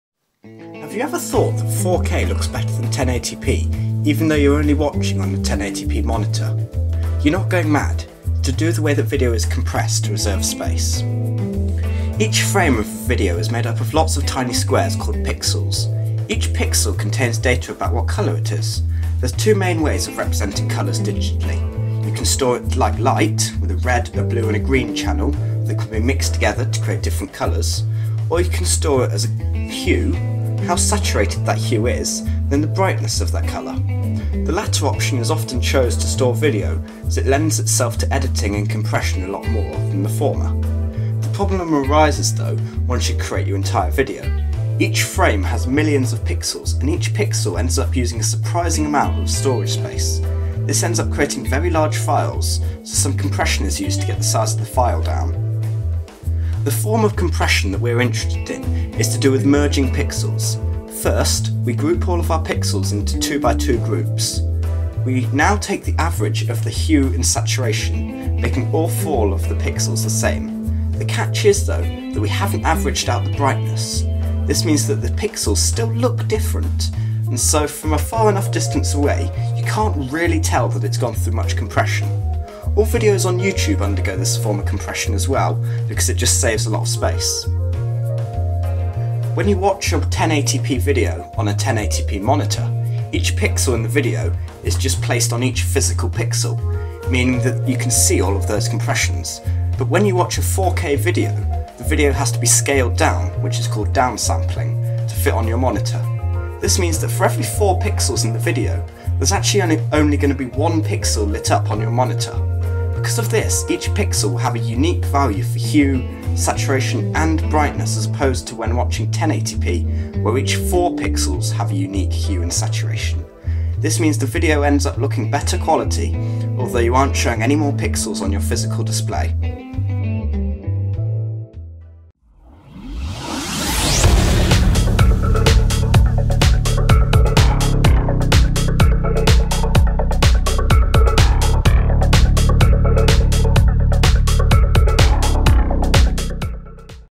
Why Does 4k Look Better Than 1080p On A 1080p Monitor?
In this video I cover one form of video compression and why it affects the quality of video when watched on the target resolution.